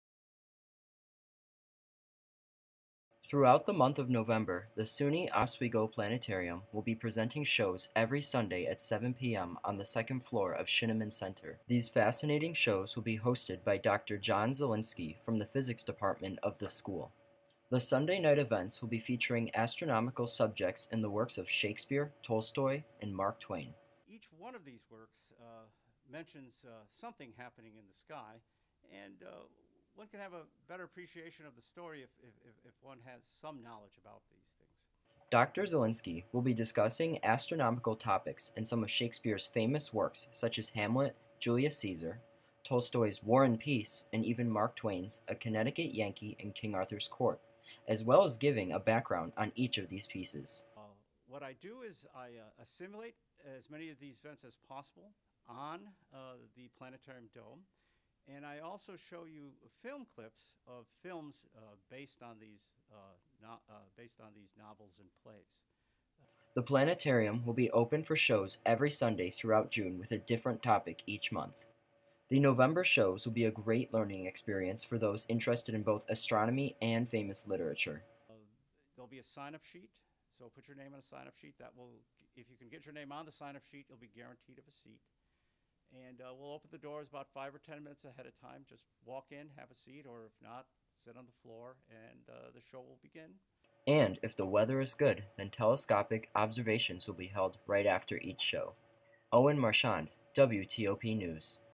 Oswego Planetarium
WTOP10-TV reporter Owen Marchand explores the Planetarium at SUNY Oswego's Shineman Center.